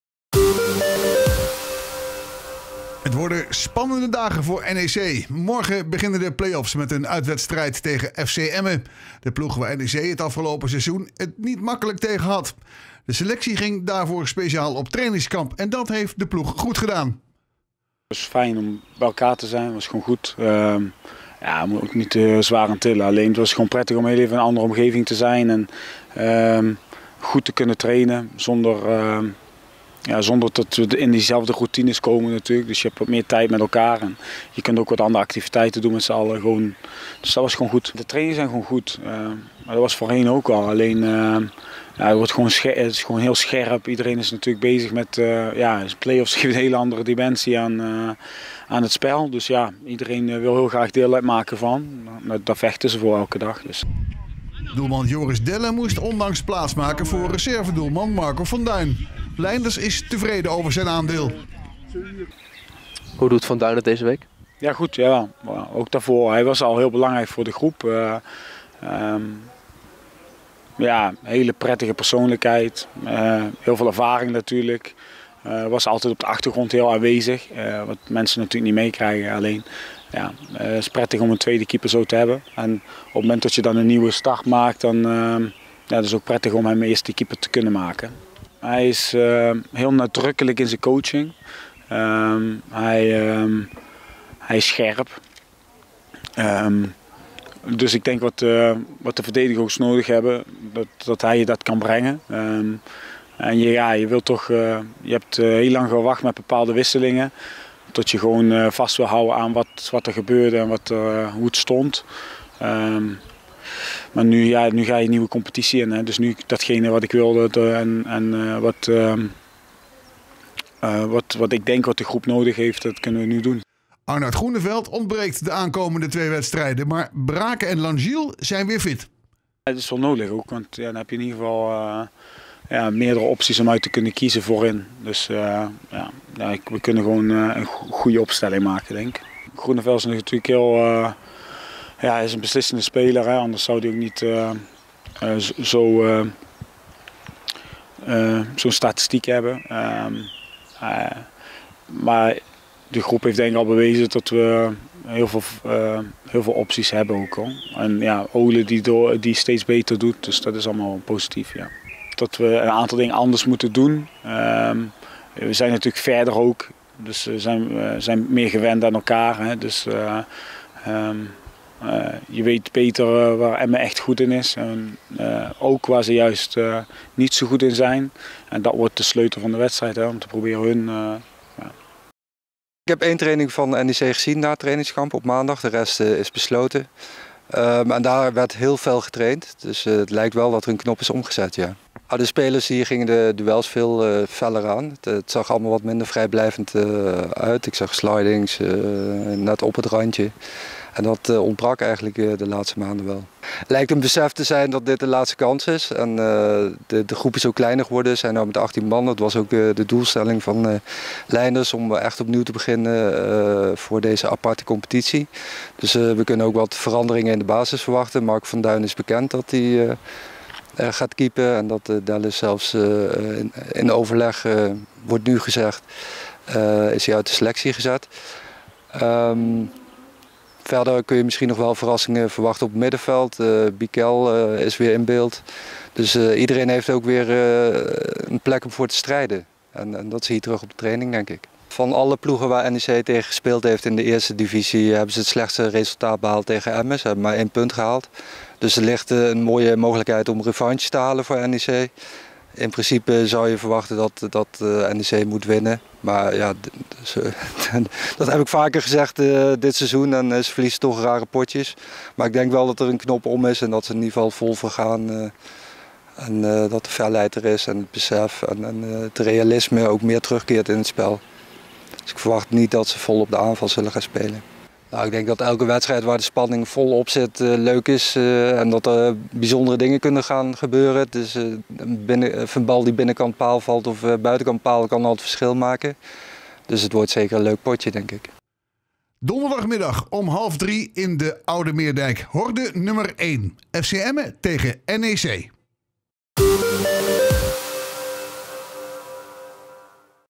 FC Emmen maakt het NEC dit seizoen moeilijk
Twaalf dagen na de deceptie in Dordrecht begint NEC donderdag aan de – hopelijk – vier duels durende epiloog van het teleurstellende seizoen in de Jupiler League. Op Hemelvaartsdag gaan de mannen van trainer Pepijn Lijnders in de play-offs op bezoek bij FC Emmen.